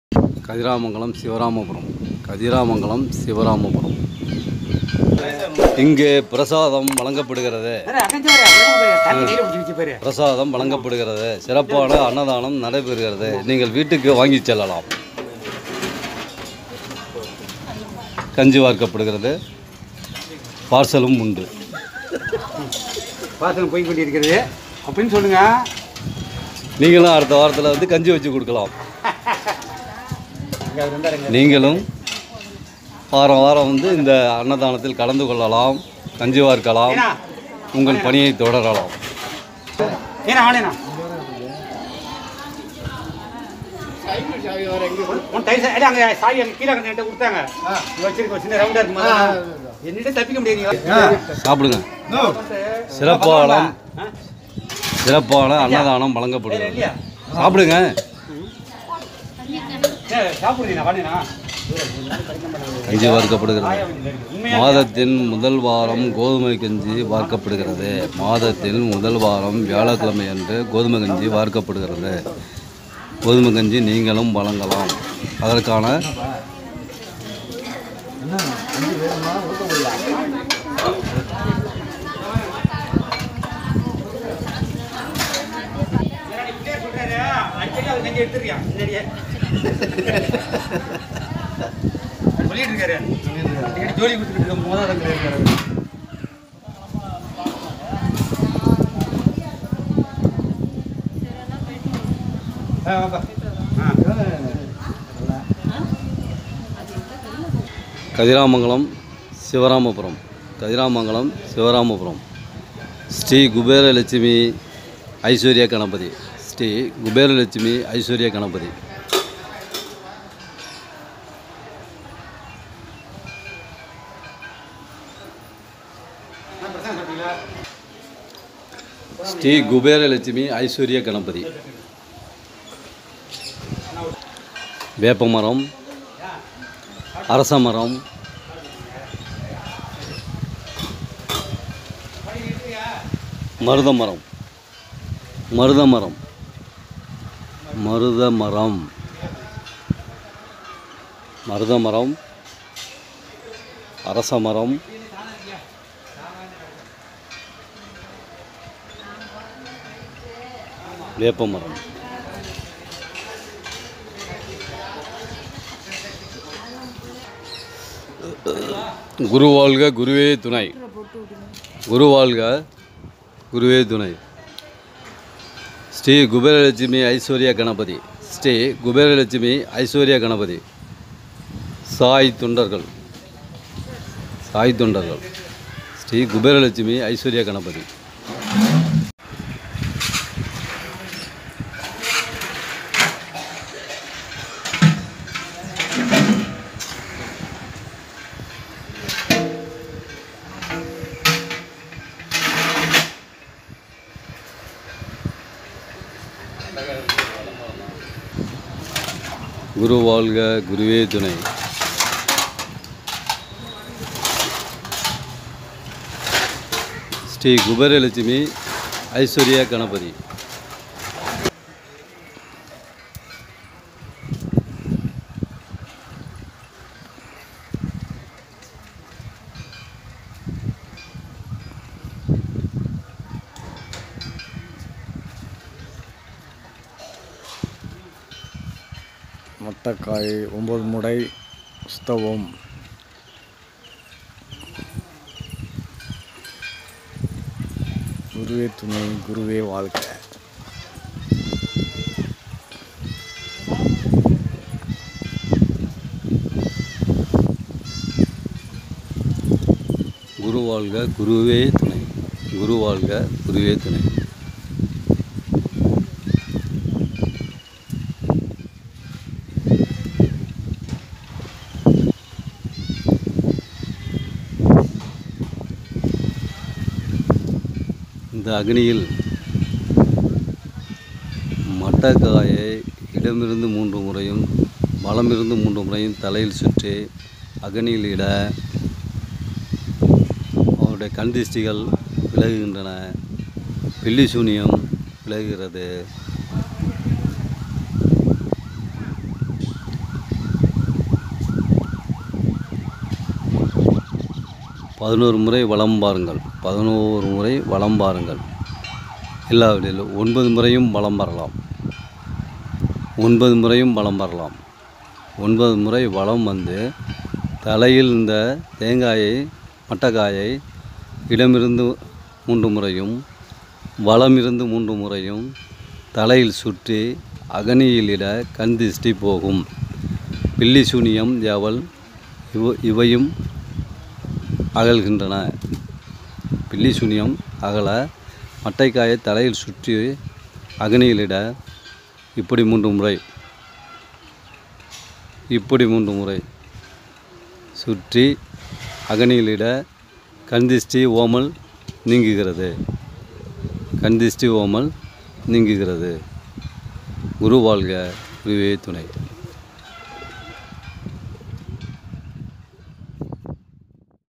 சிவராமபுரம்,ஶ்ரீ ஷீரடி சாய்பாபா மஹாராஜ் ஆலயம்,ஒவ்வொரு ஒரு வியாழக்கிழமை தோறும் கஞ்சி வார்க்கப்படும்.
ஶ்ரீ ஷீரடி சாய்பாபா மஹாராஜ் திருக்கோயில்,ஒவ்வொரு ஒரு வியாழக்கிழமை தோறும் கஞ்சி வார்க்கப்படும்.கதிராமங்கலம். சிவராமபுரம். மயிலாடுதுறை...